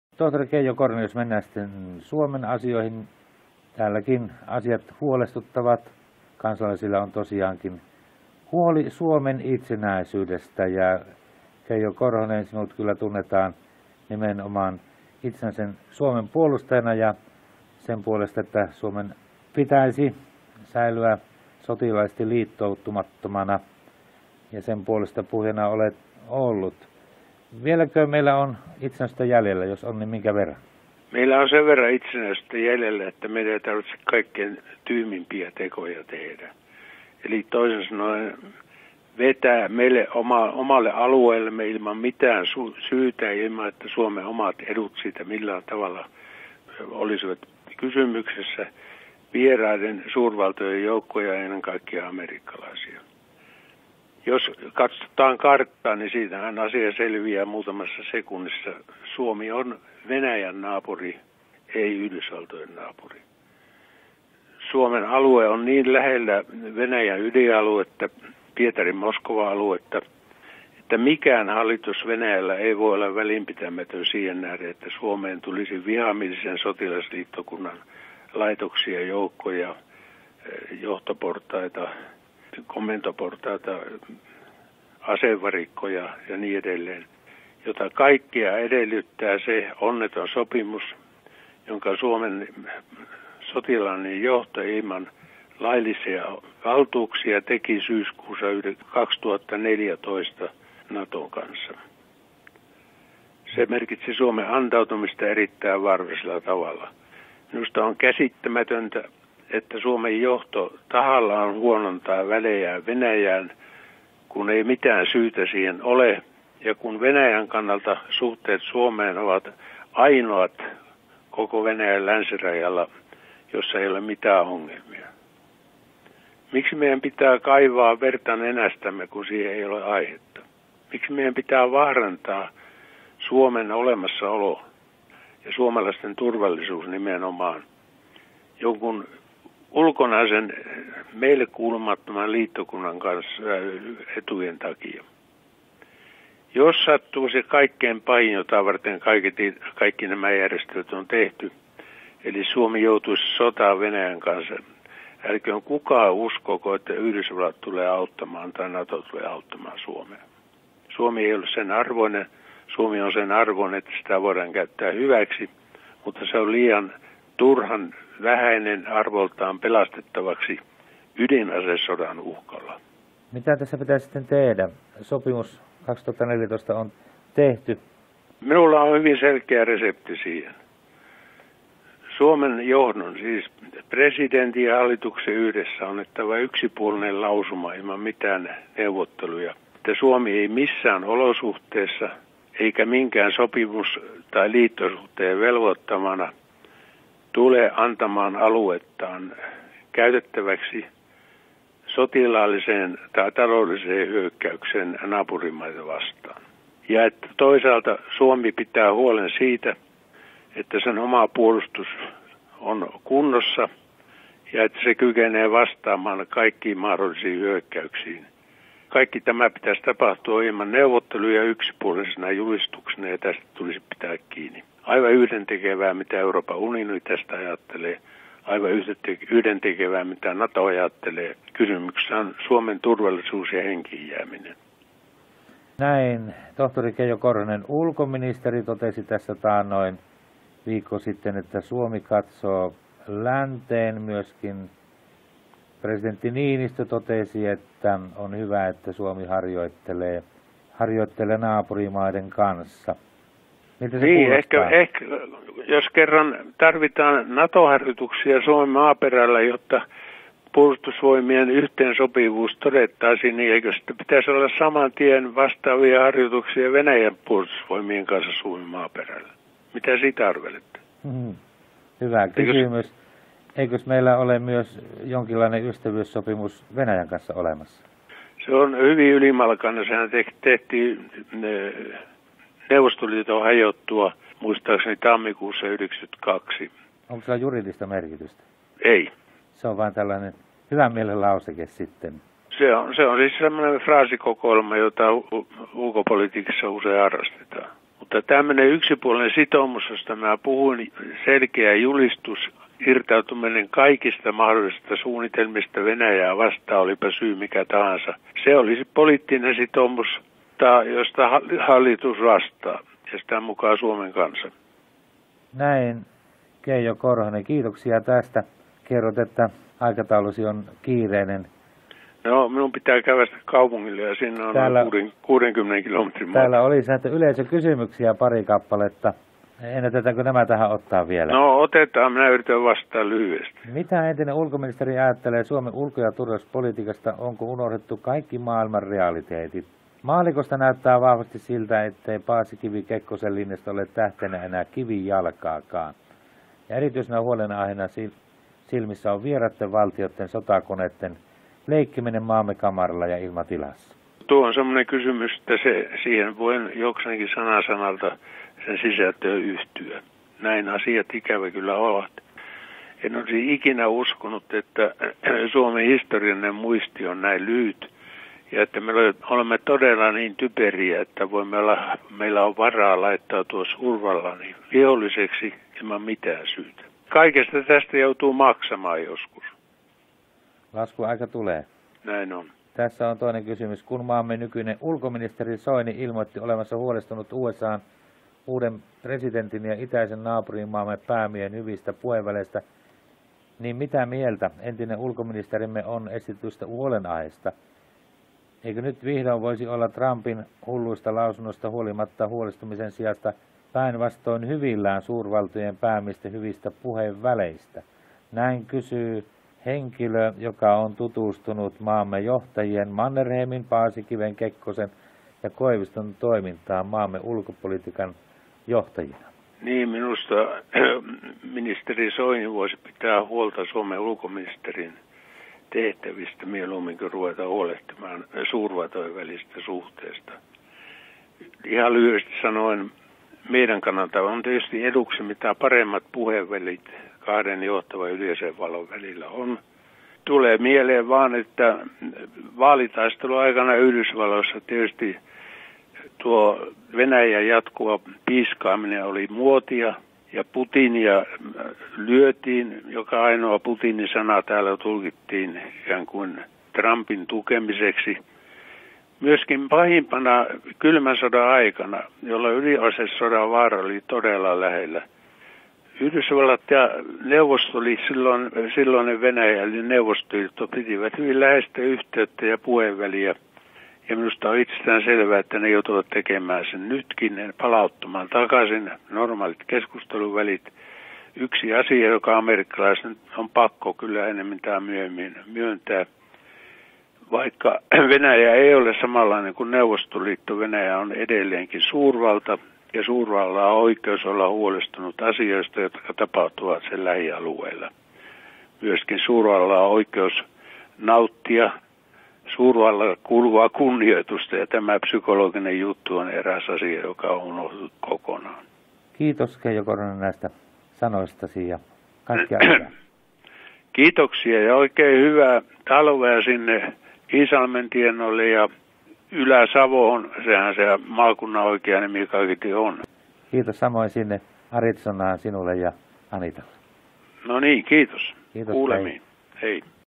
Keijo Korhonen 17.11.2016 0sa 2
Entisen ulkoministerin tohtori Keijo Korhosen terveiset Arizonasta.

Linkissä mielenkiintoinen haastattelua tältä toisinajattelijalta.